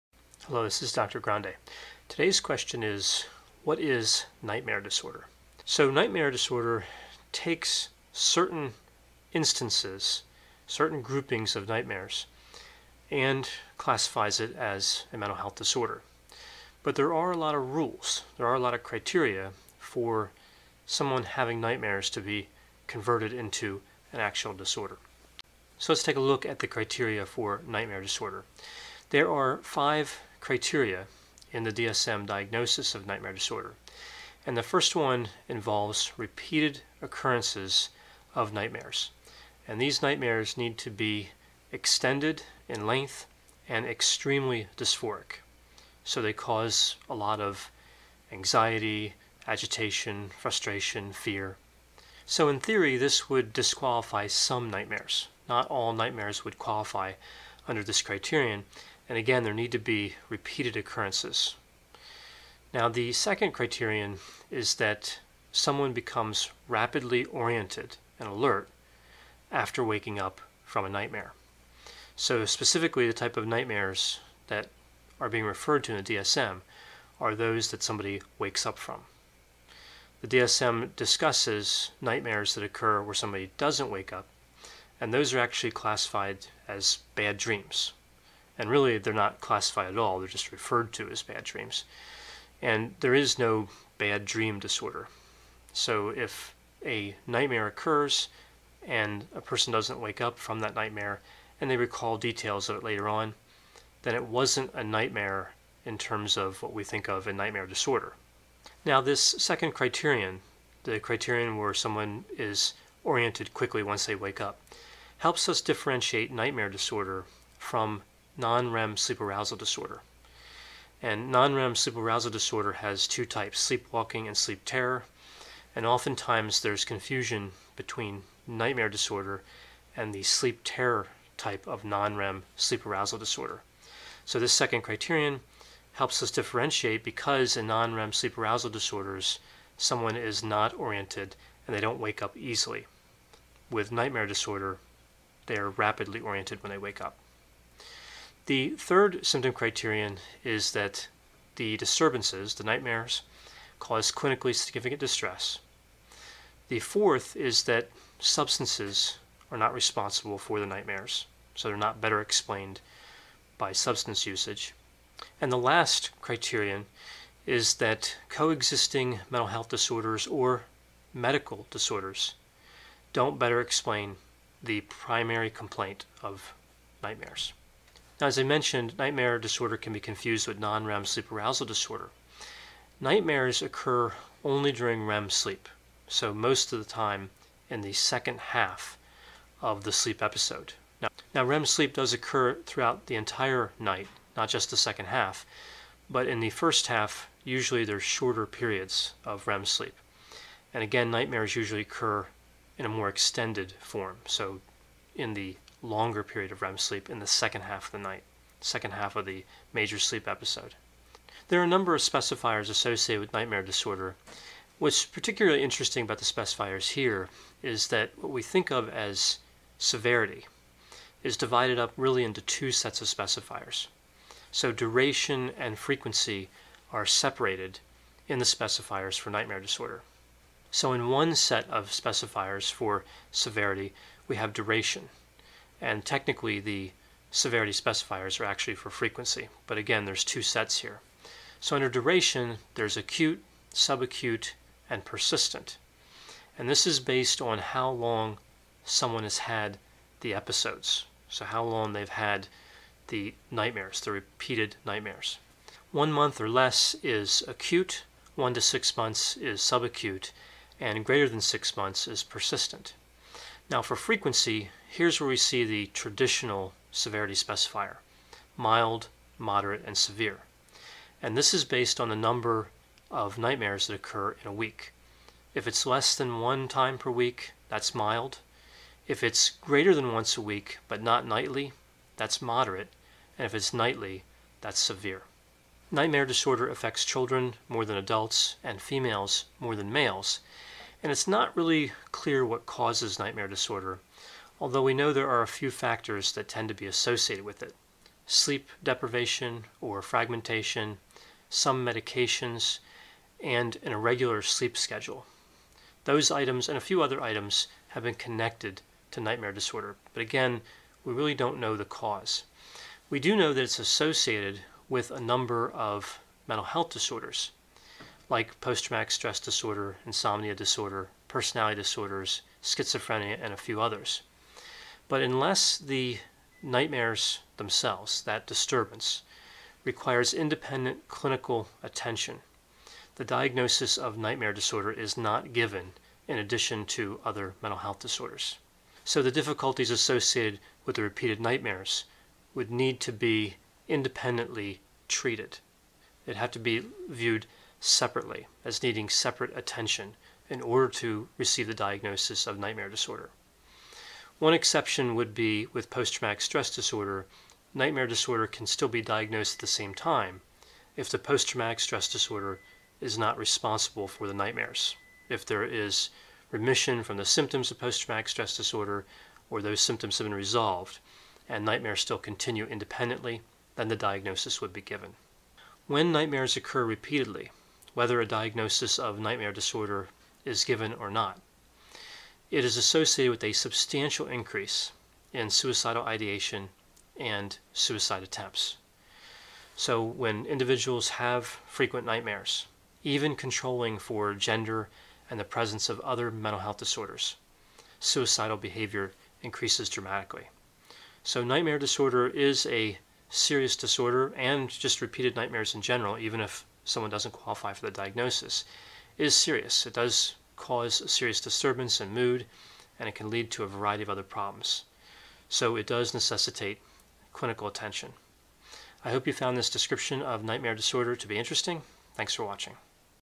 What is Nightmare Disorder?
This video describes Nightmare Disorder. Nightmare Disorder is characterized by repeated occurrences of nightmares, rapid orientation upon waking, and clinically significant distress. Sleep deprivation and fragmentation may contribute to the development of Nightmare Disorder. Nightmares are a type of dream that wakes up the person having the nightmare. They are fairly common and people usually remember the details of a nightmare when awakened by it. Nightmares occur during REM sleep. They are more common in women than in men, and more common in children than adolescents. NREM Sleep Arousal Disorder is a distinct disorder and is associated with slow orientation upon waking up.